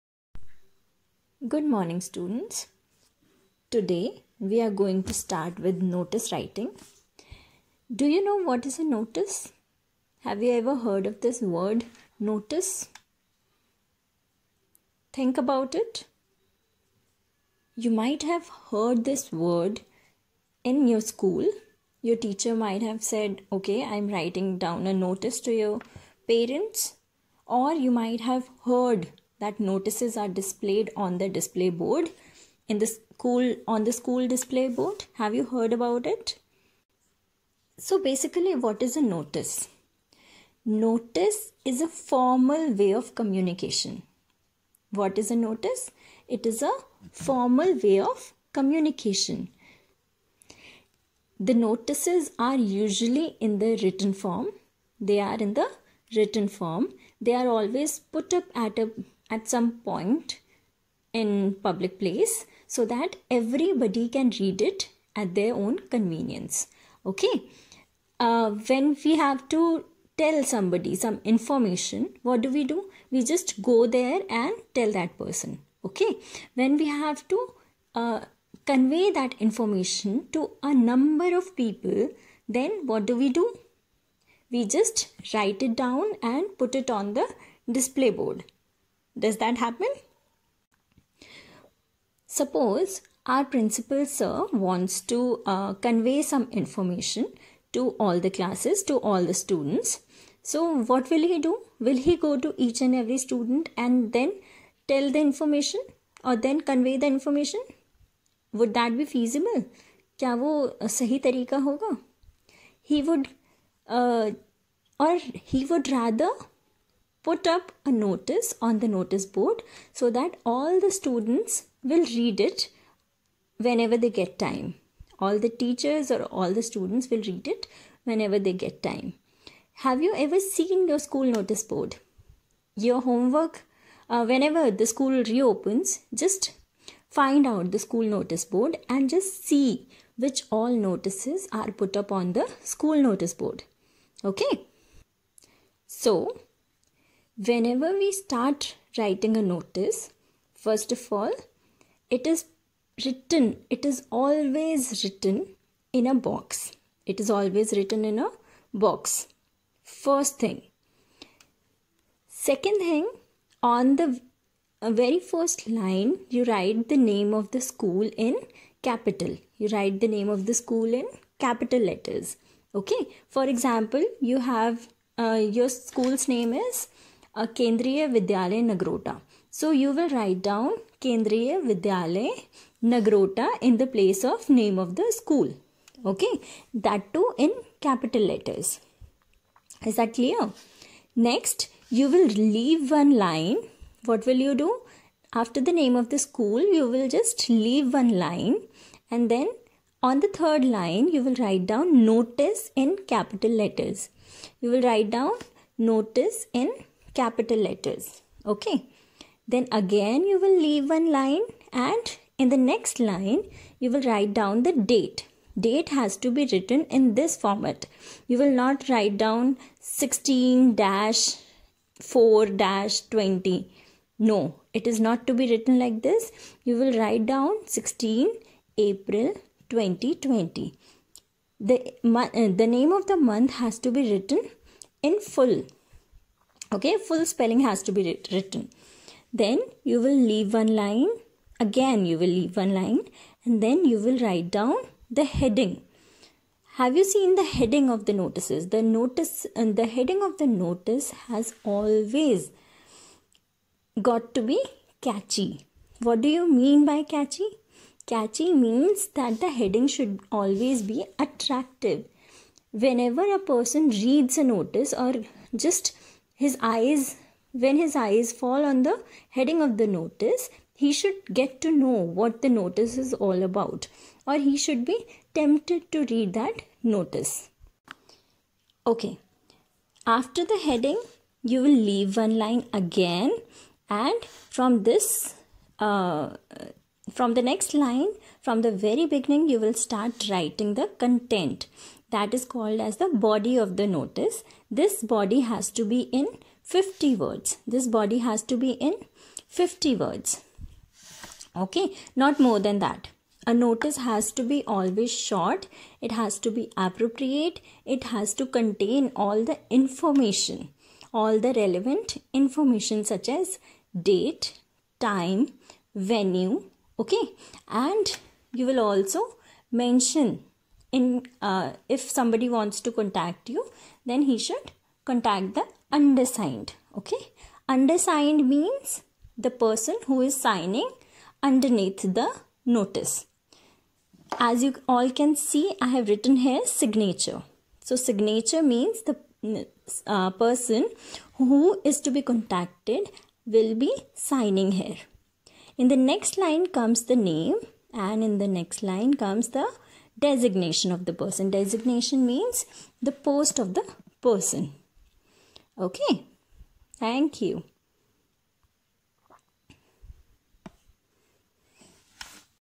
CLASS-6TH Notice Writing
The video discusses about Notice Writing for school children